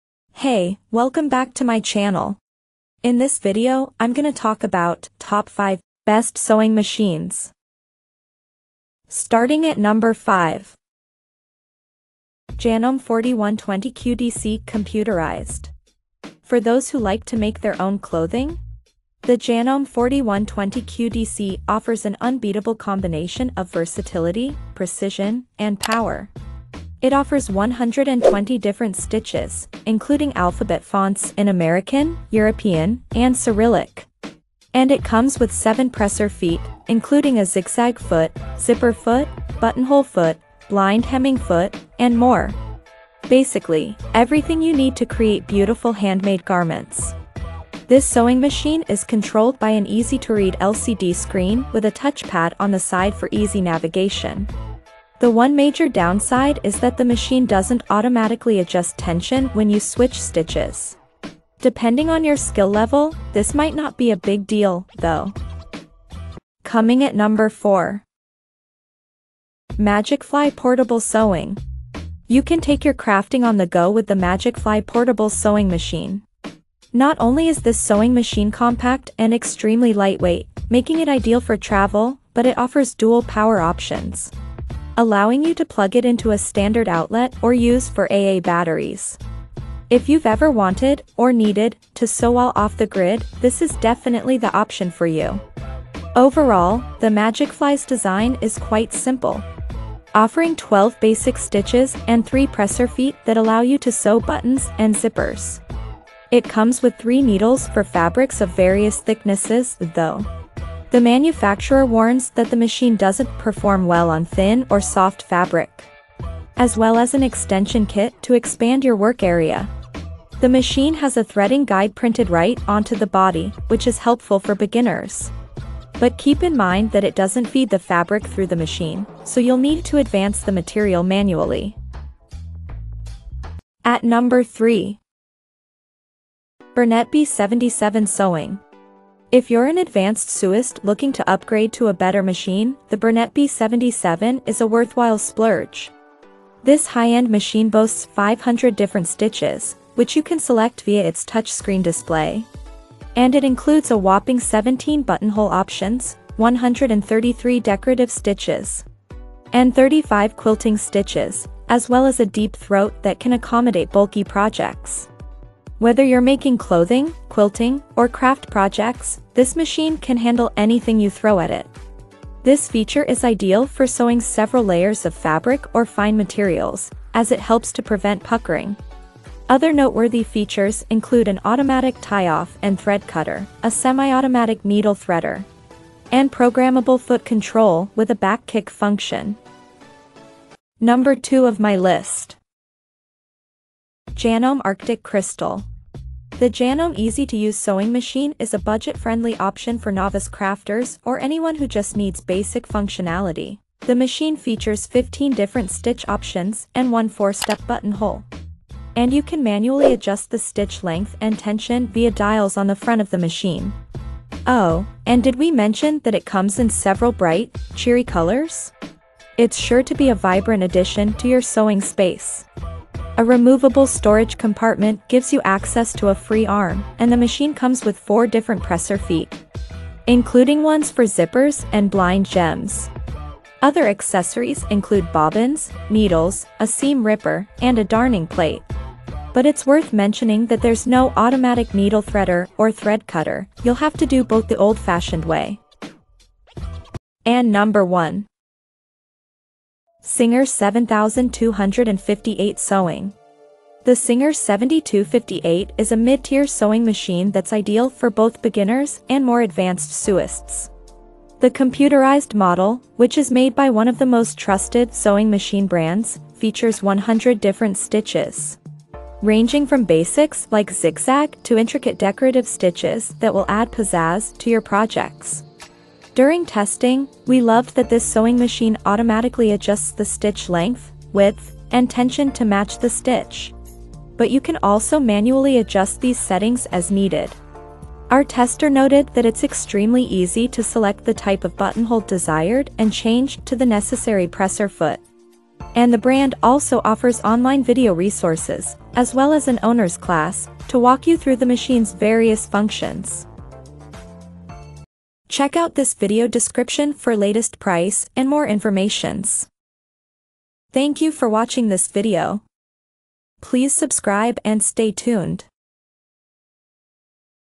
Top 5 Best Sewing Machines in 2022
► Here is the Updated Prices of Top 5 Best Sewing Machines in 2022!
1. SINGER | 7258 Sewing

2. Janome Arctic Crystal

3. Magicfly Portable Sewing

4. BERNETTE B77 SEWING

5. Janome 4120QDC Computerized